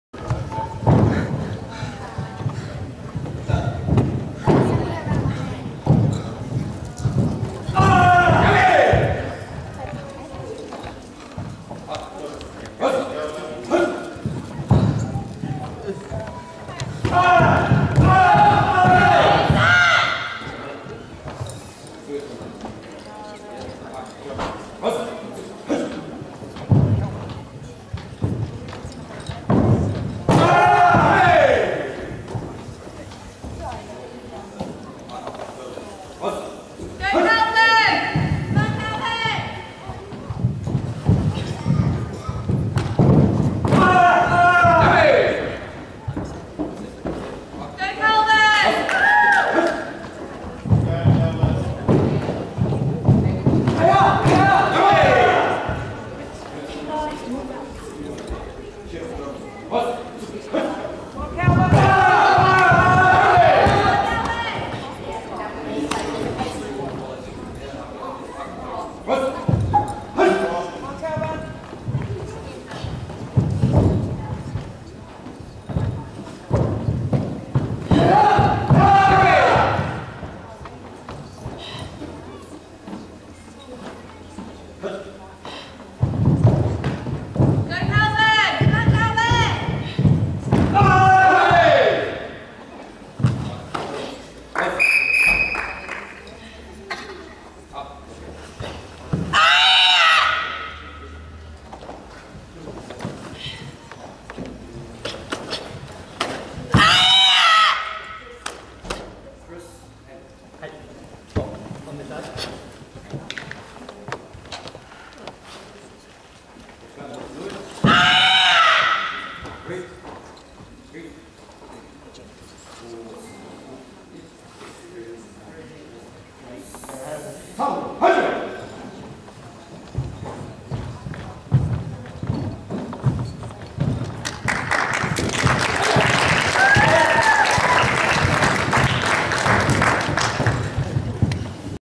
gkr chch r2 05 09 2015 rg1 jnr open female kata 03c seisan
Phillipstown

05 September 2015

Ring1 Junior Open Female Kata & Kumite